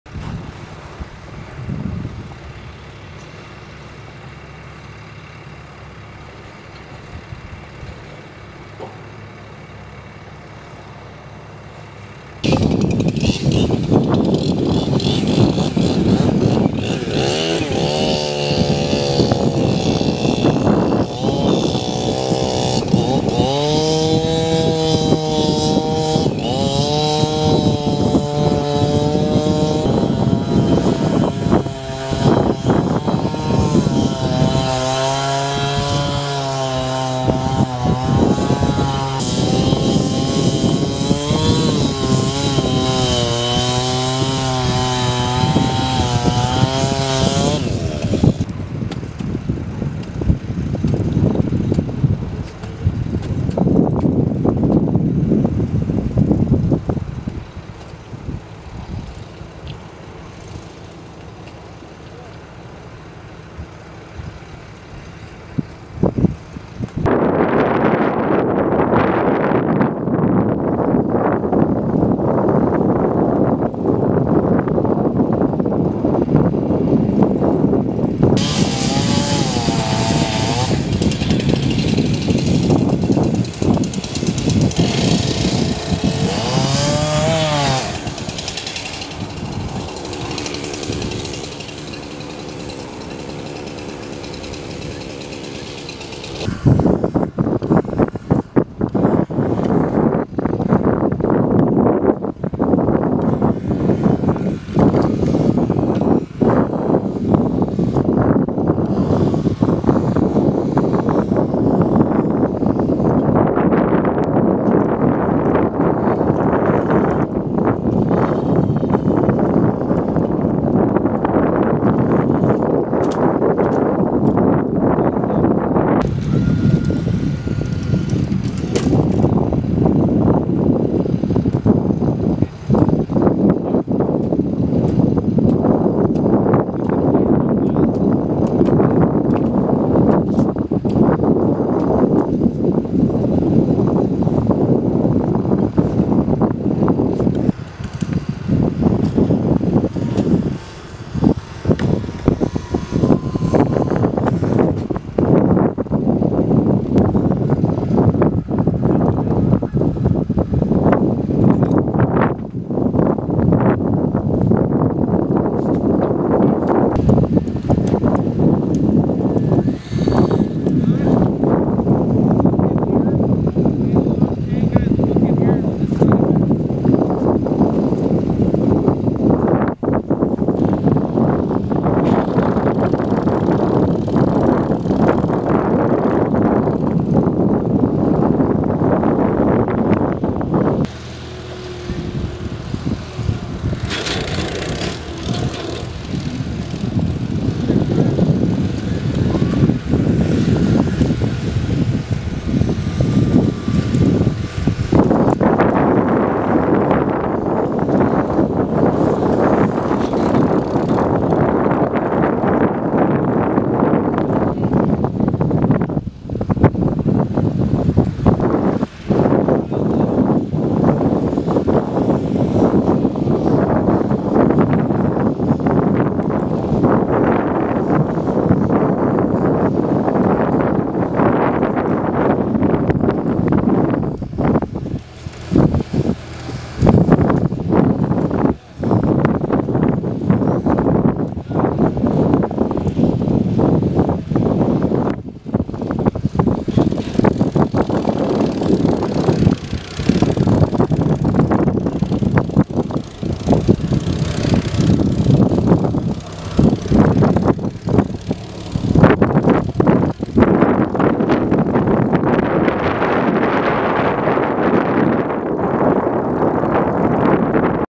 Rice Lake, WI Mini Van Being Pulled Out After Breaking Through Ice - 1/3/2017
This video shows the van that dropped through the ice on Rice Lake back on 12/16/2016 being pulled out today. Ice conditions were safe enough to allow for the removal of the van today.
Shot Description

Clip 1. Shows the crew setting up to pull the van out of the lake.
Clip 2 through 4. Shows Someone cutting the ice around the van with a chainsaw.
Clip 5 through 7. A diver hooking up chains to the van.
Clip 8-9. Crew starting to lift the van from the lake.
Clip 10. Crew removing some ice from the van. The front chain let go and the front end of the van dropped down.
Clip 11 through 13. The crew lifting the van once again.
Clip 14. Crew has the van out of the water.
Clip 15. The problem chain let go once again. Front end of the van dropped back into the water.
Clip 16. Van is lifted back out of the water.
Clip 17. The winch setup and van being pulled away from the open water.